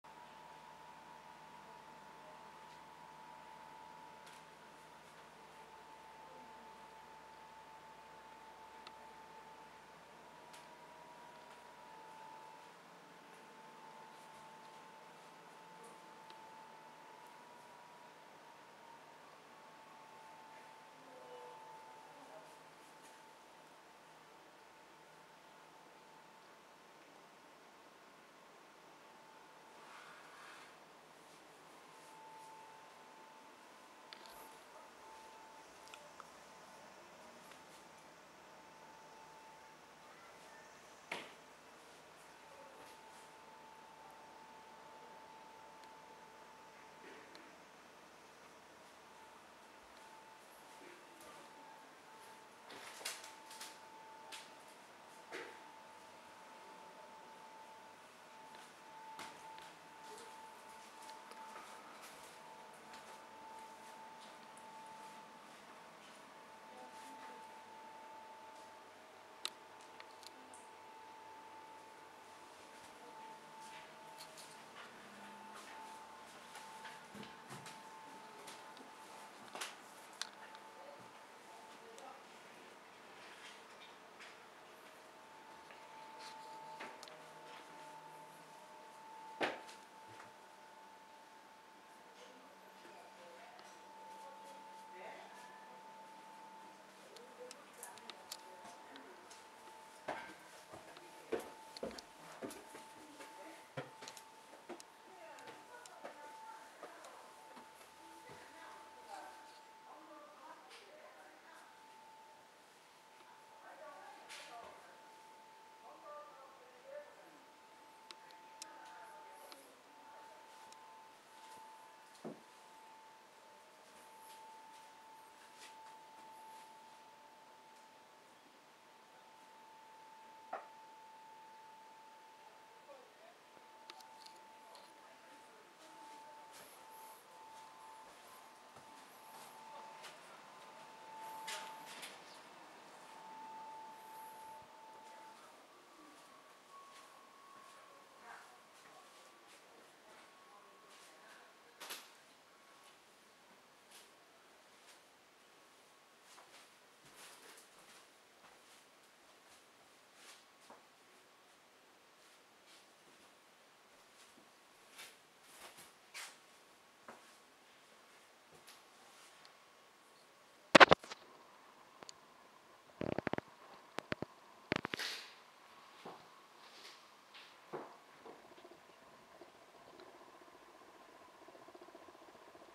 二階堂上之庄一戸建て♪賃貸のマサキ
天理市二階堂にあります4LDKアパートの【二階堂上之庄一戸建て】の紹介動画になります。